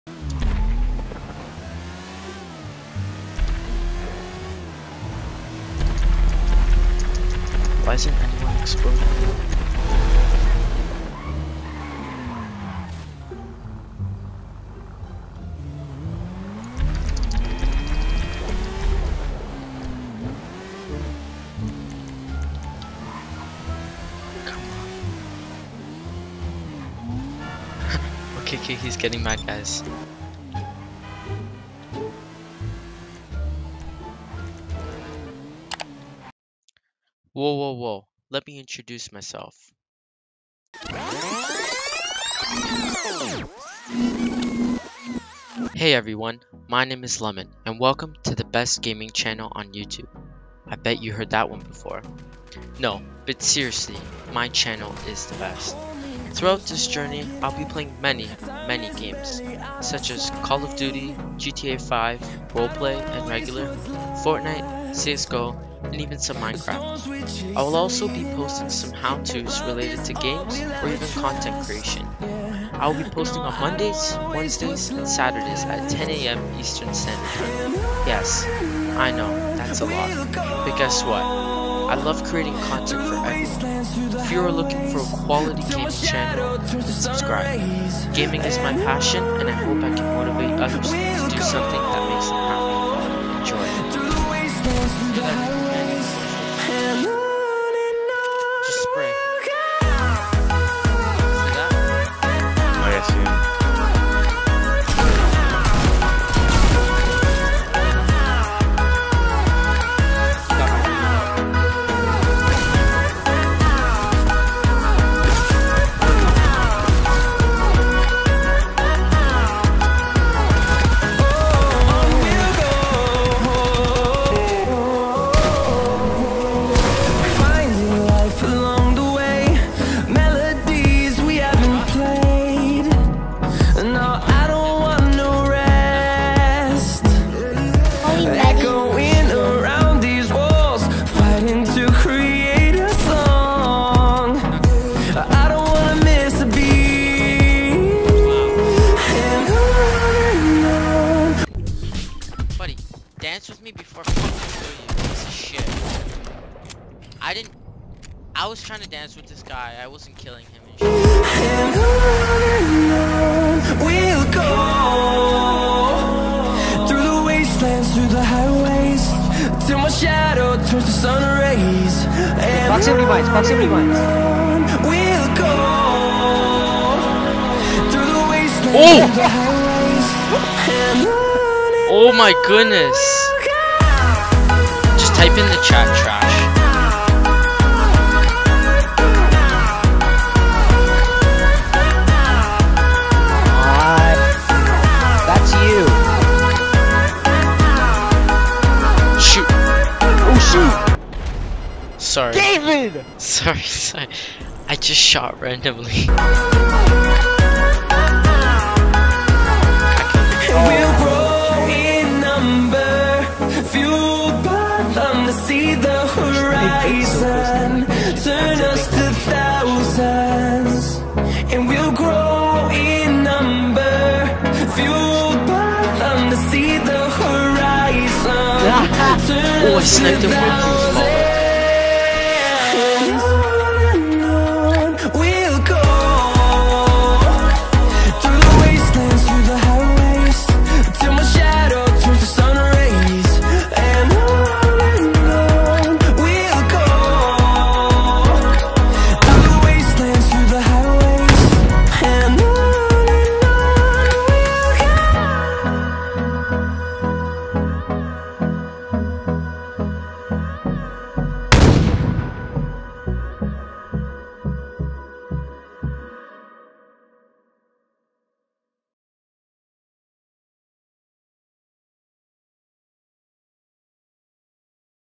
LEMON | OFFICIAL WELCOME VIDEO | MONTAGE
Hey Everyone! How are you all doing? This is my official Welcome Video, I hope you all enjoy this as Prophet and I have been working very hard to get this video out. This video has been in my mind since I restructured the channel, I hope you enjoy it as much as I did making it.

BIG THANKS TO PROPHET FOR EDITING MY VIDEO!